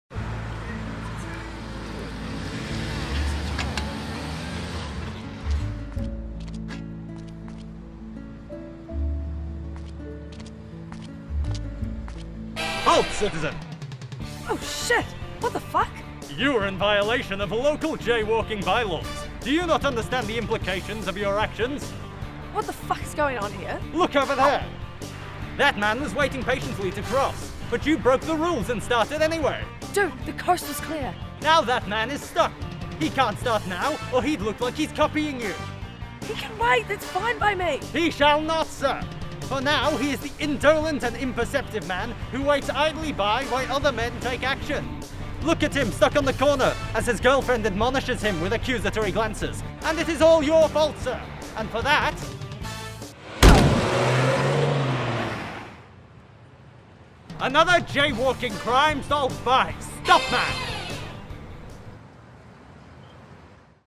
Pedestrian Crossing - Capping Show 2016 Video Sketch
Video Sketches from the 2016 OUSA Capping Show, MAKING GRAD.

Video filmed and edited by the team at Bus of the Undead Productions.